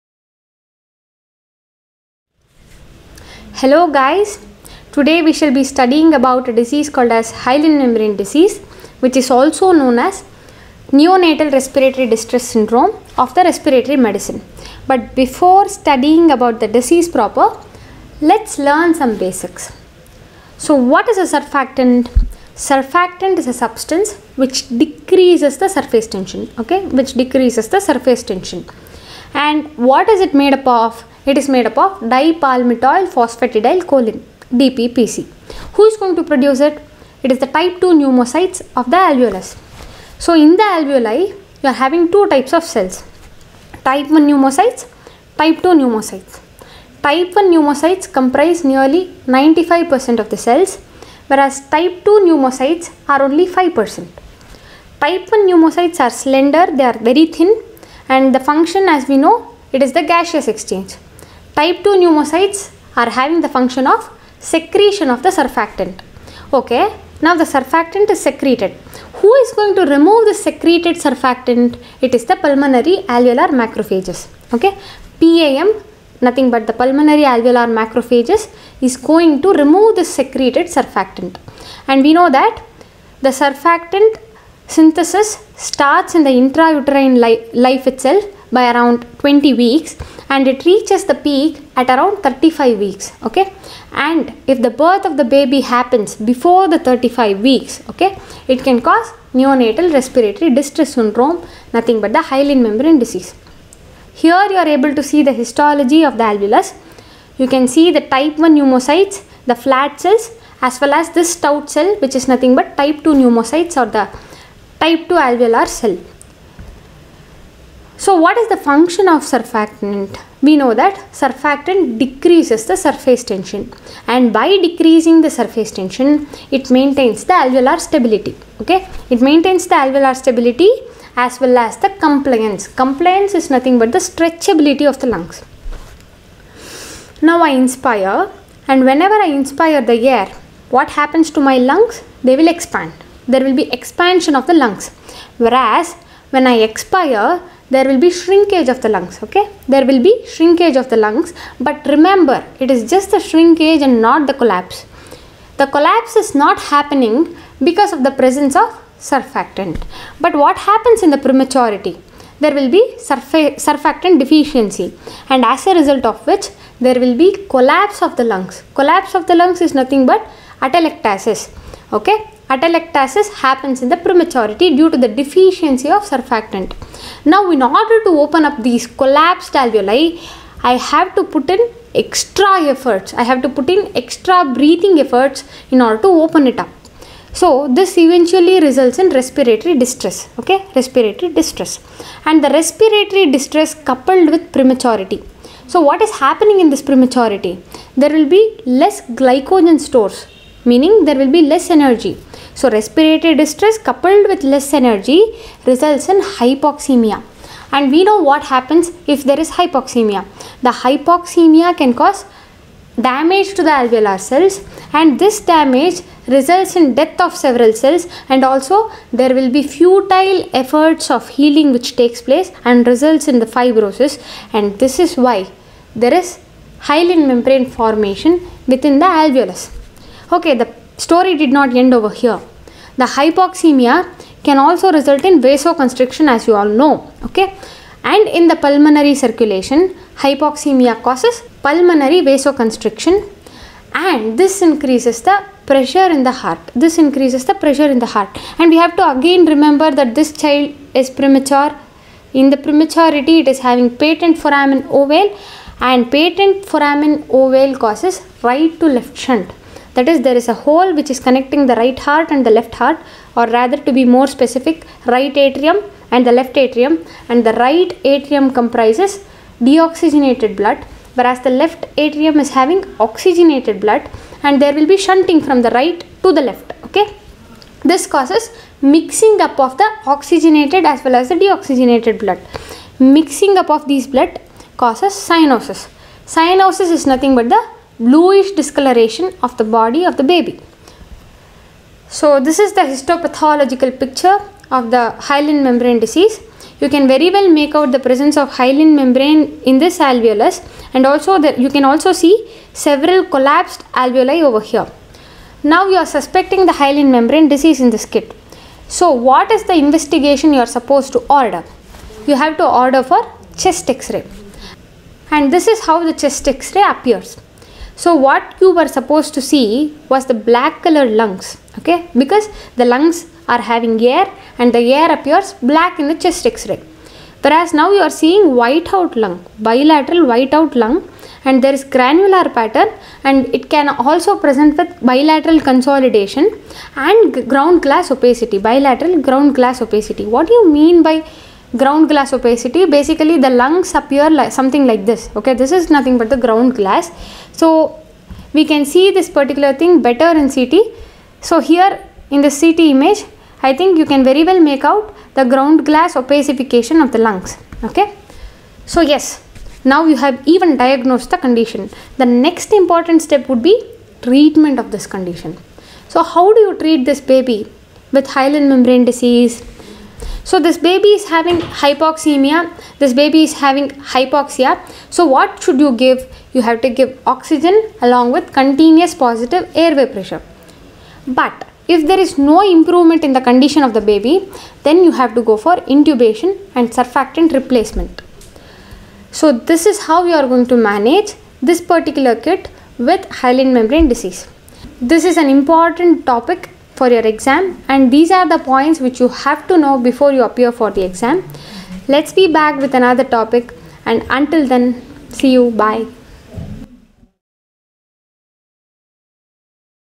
Hyaline Membrane Disease | Neonatal Respiratory Distress Syndrome
Hey peeps!

I have made a humble attempt of making this video! Here. If you want me to take up any topic in particular, please write the name of the topic in the comments section below... I will definitely take them :)

This video is mainly done to make people fall in love with this topic and have very little time to go through the details of everything. With this knowledge one can definitely solve all the questions pertaining to Hyaline membrane disease a.k.a Neonatal RDS.

Happy learning!!

I'd like to thank my friend Himadrish for helping me make this video :D